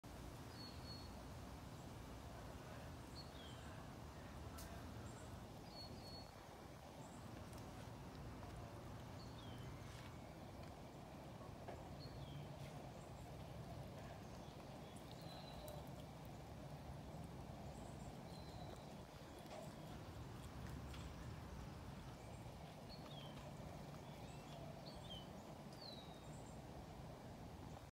Squirrel Loves Eating Corn On His Squirrel Picnic Bench 😅
This squirrel loves eating corn on his picnic bench!